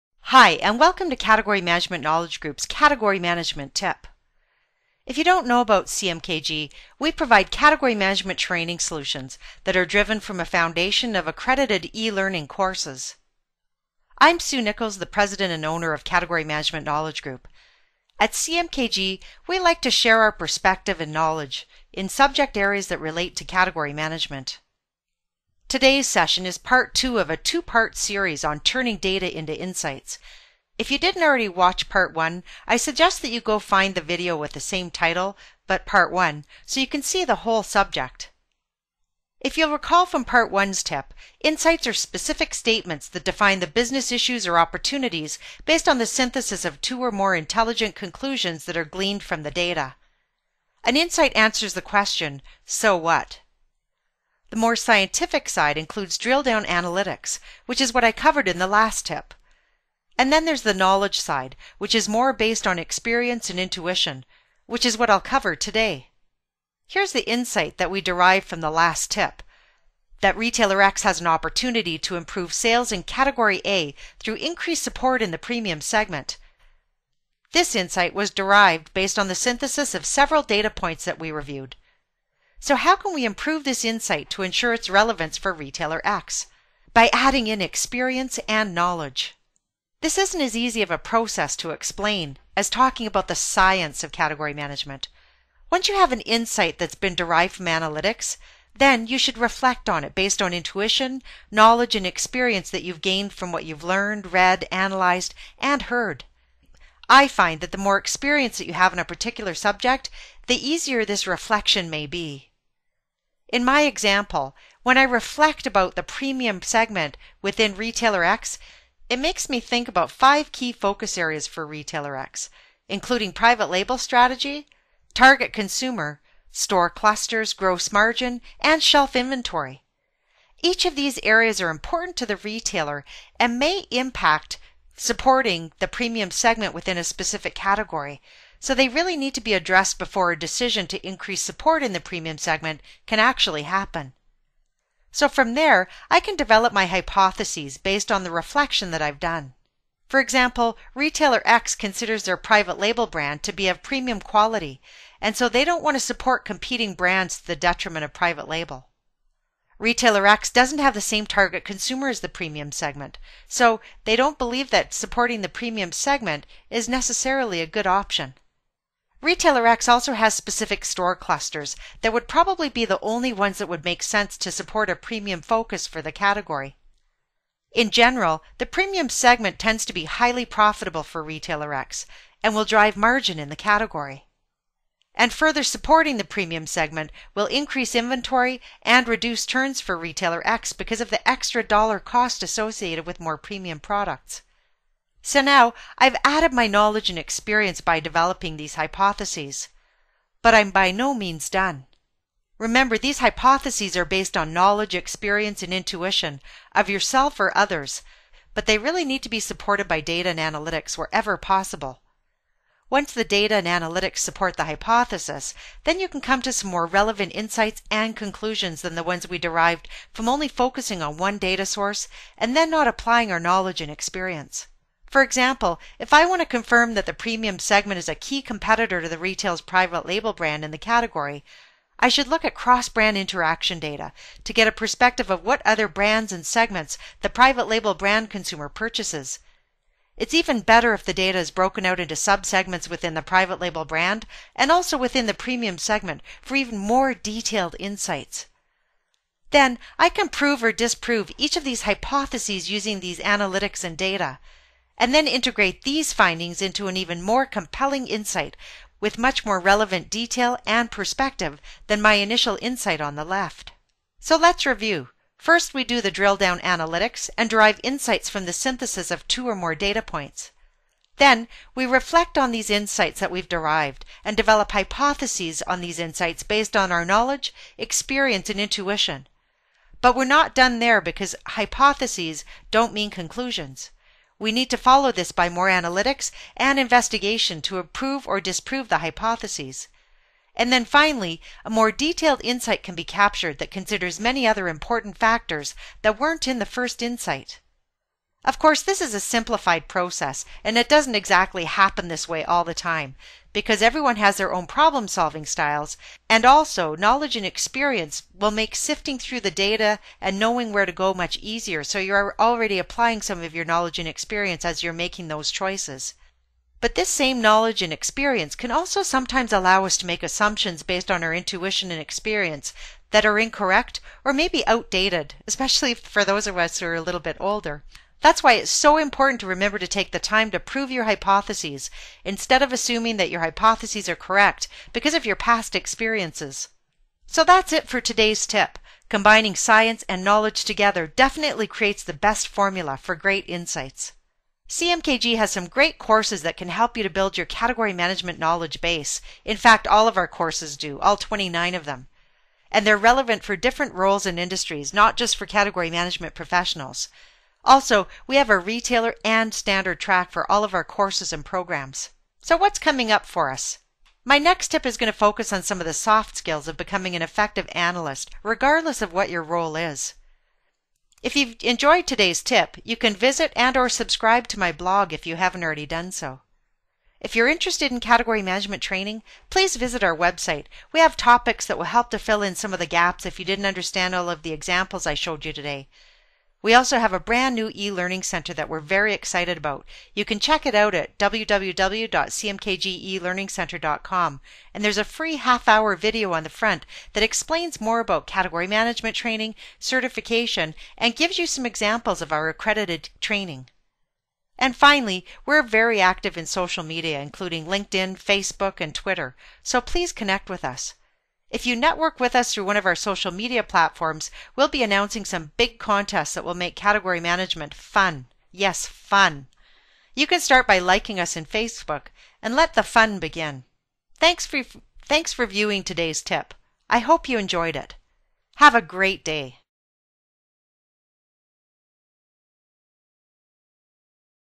Category Management Series Tip #15: Turning Data Into Insights
In this 2-part Category Management tip, we show you how to turn data into observations, and then into insights. Part 1 walks you through the drilldown analytics; and this Part 2 video talks about the importance of experience and intuition to create more relevant and compelling insights.